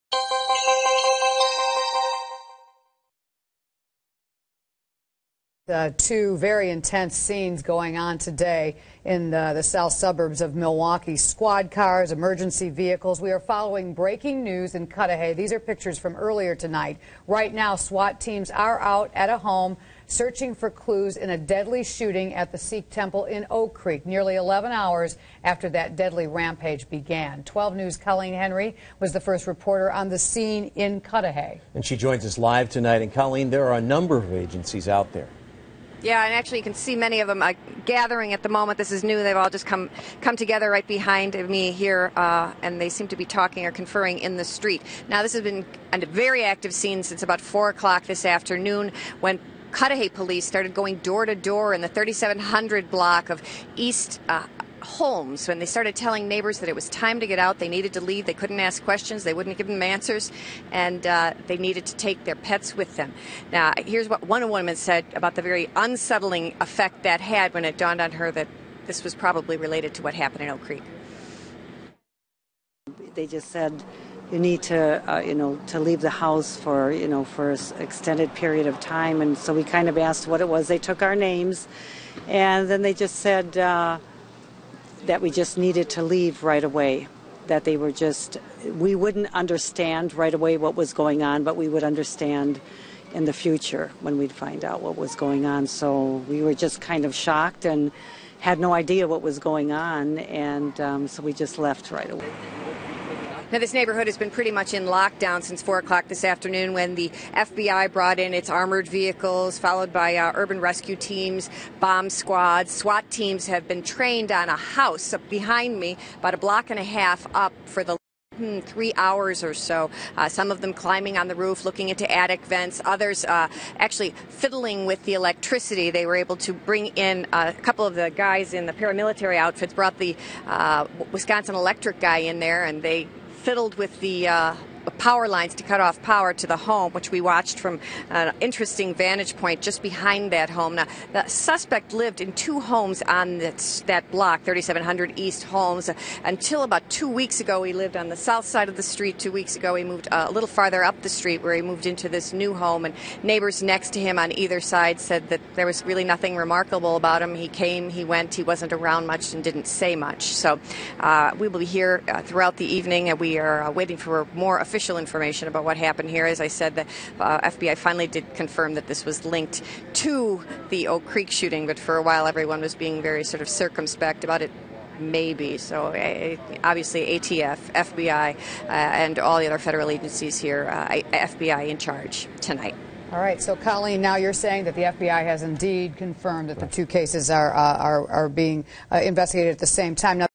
FBI: Cudahy investigation related to shooting
Agents with the FBI have told WISN 12 News that the investigation focused on a home in Cudahy is related to a fatal shooting in Oak Creek earlier in the day.